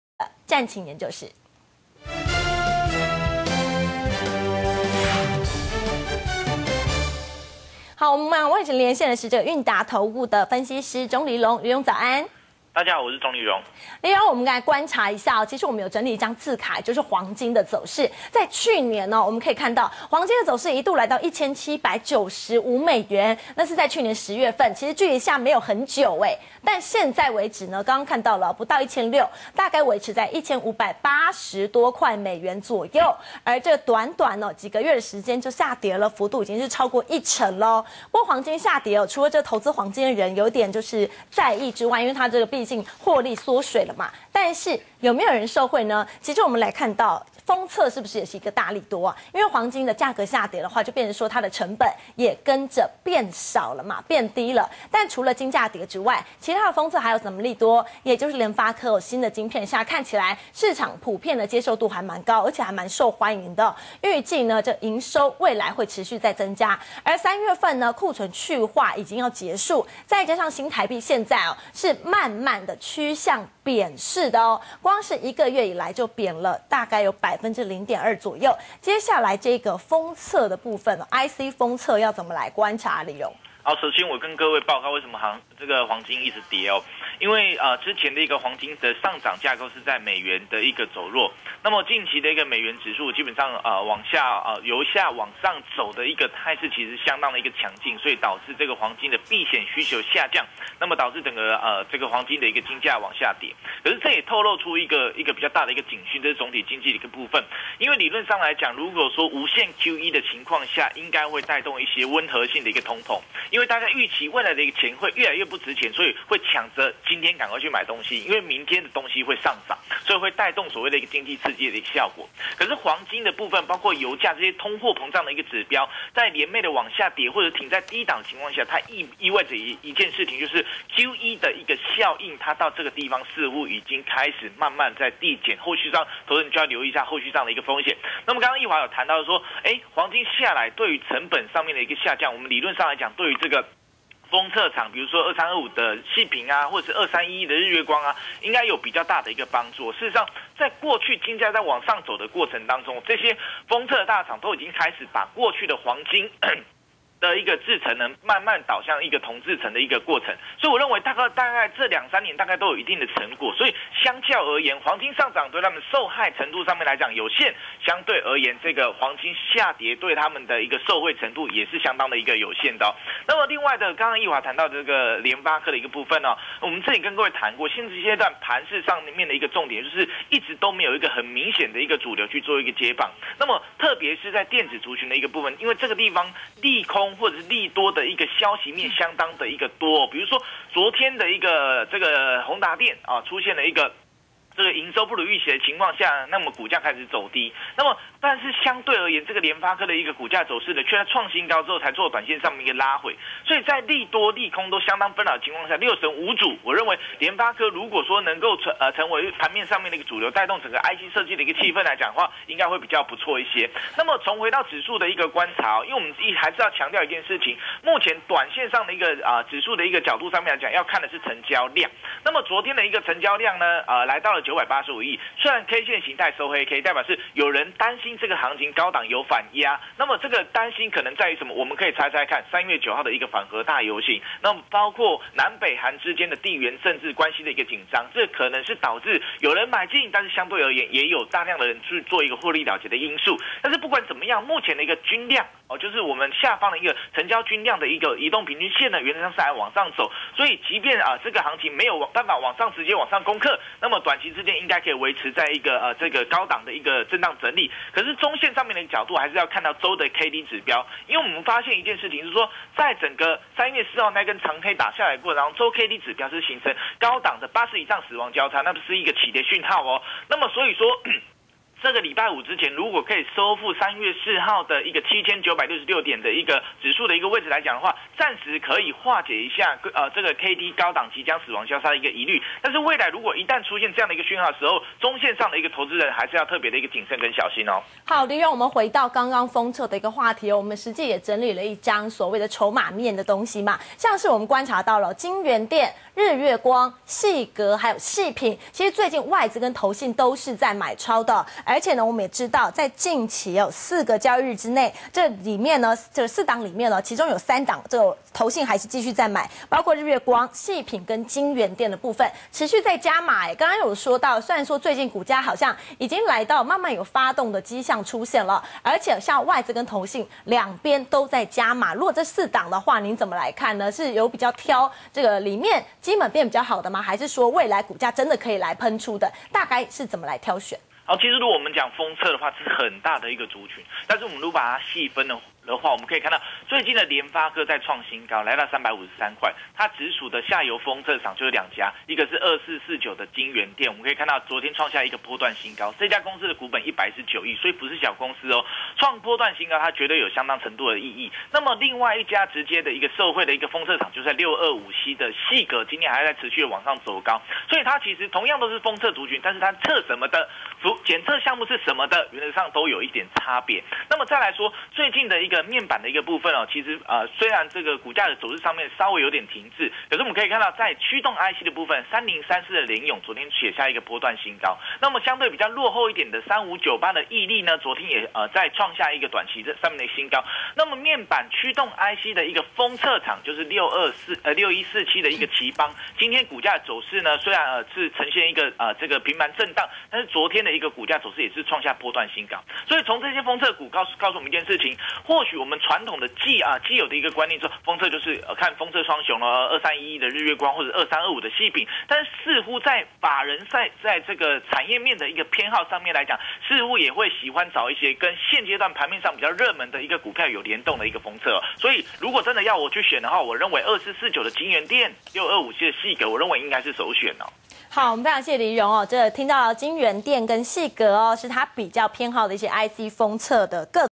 國際金價跌 台封測廠利多high | 錢滾錢 | 三立財經台CH88
封測業景氣回溫愈趨明顯，近期國際金價下跌及新台幣走貶雙利多加持，法人預估，隨第2 季訂單增溫，包括日月光、矽品等封測大廠第2季毛利率同步看升。